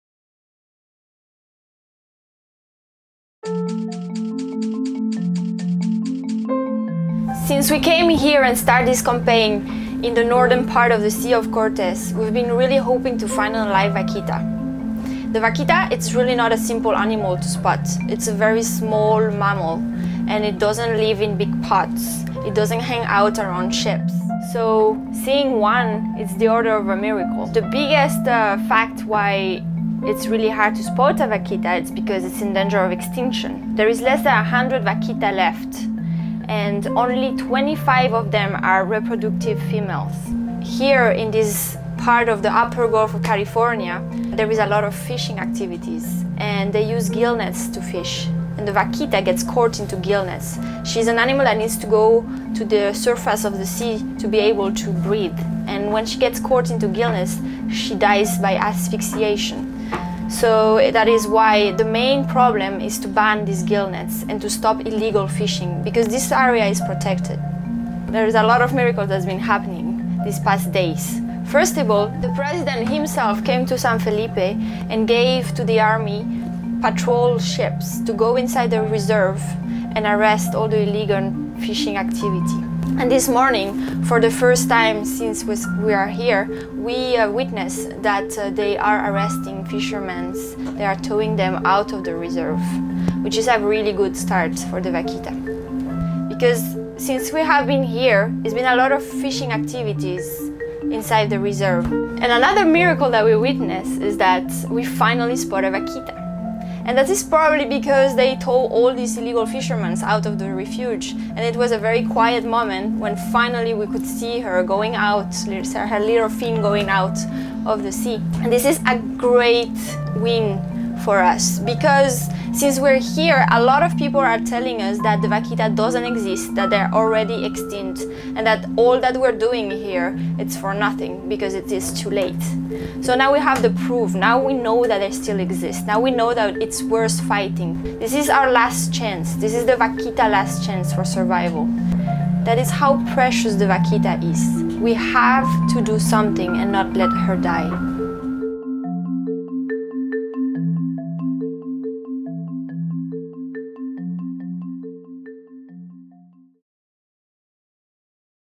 Sea Shepherd Captures Rare Footage of Elusive Vaquita During First Sighting since 2013
On April 18, Sea Shepherd Conservation Society captured rare footage of the elusive and endangered vaquita porpoise in the waters of Mexico’s Gulf of California, the small cetacean’s only home on Earth. The sighting marks the first time since 2013 the shy creature has been spotted and filmed in the Sea of Cortez. The vaquita documented by the crew of Sea Shepherd USA’s Operation Milagro (Operation Miracle) campaign is one of only 97 remaining members of its species, considered by many to be the rarest marine mammal in the world.